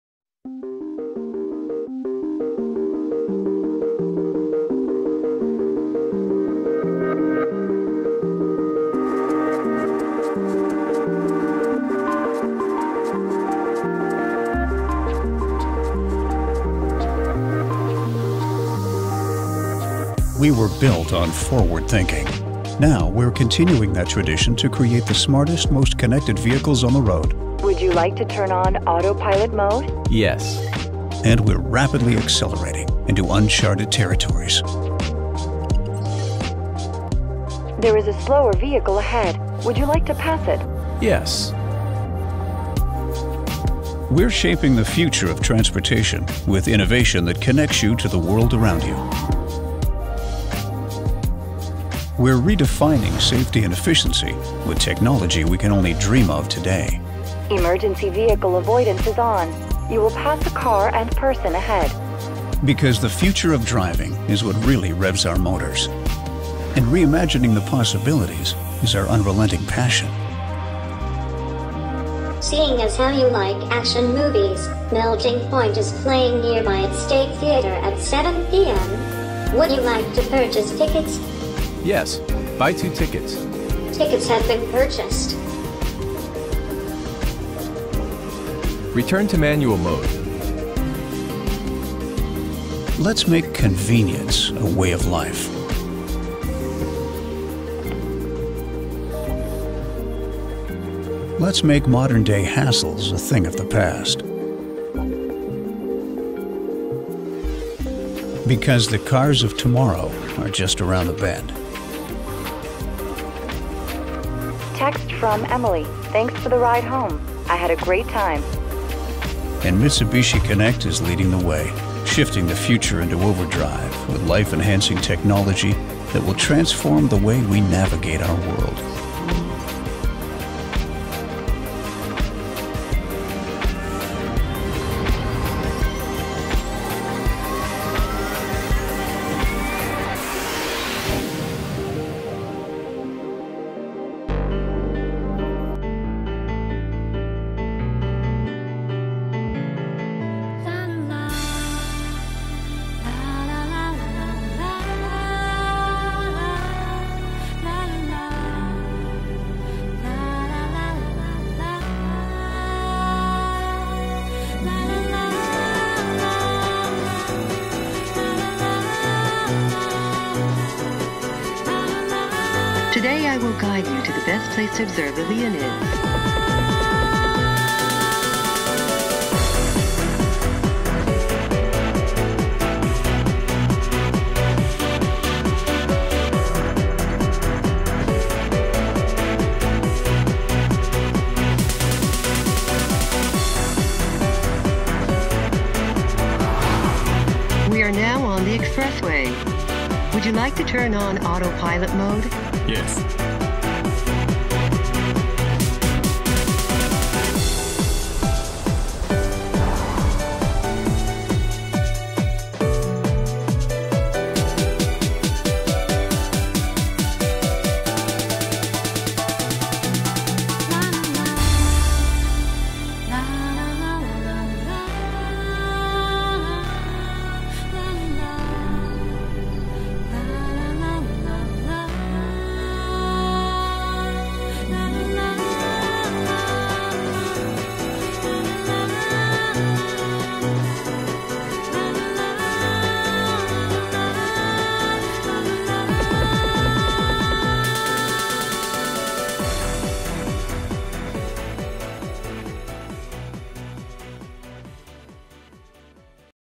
Mitsubishi eX Concept
System napędu elektrycznego eX Concept został wyposażony w akumulatory o wysokiej wydajności i niewielkie, ale mocne silniki elektryczne, dzięki czemu cały układ jest teraz lżejszy i sprawniejszy. Dzięki dalszym zabiegom obniżającym masę pojazdu, udało się uzyskać zasięg samochodu wynoszący 400 km bez konieczności ładowania akumulatorów.